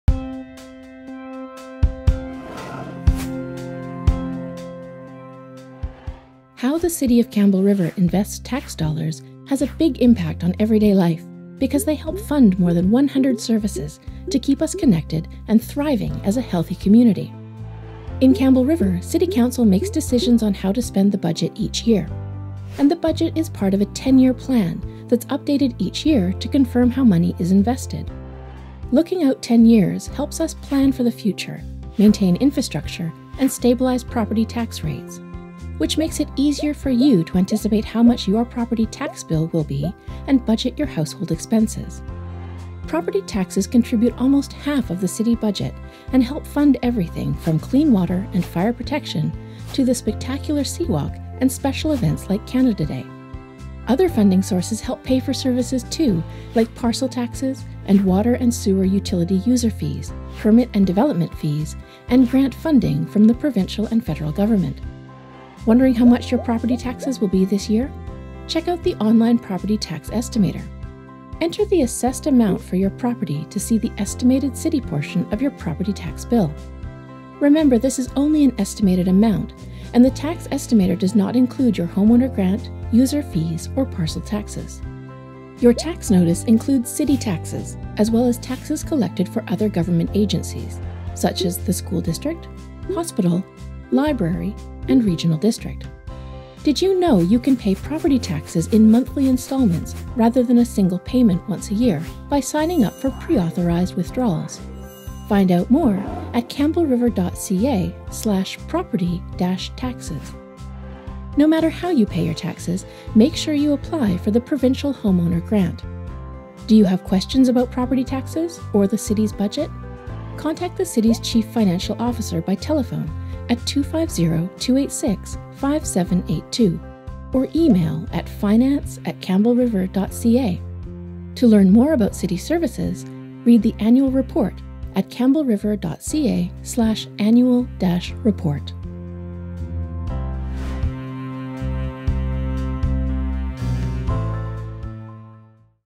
City Property Taxes
How City of Campbell River property taxes support more than 100 local government services and link to the award-winning 10-year financial plan.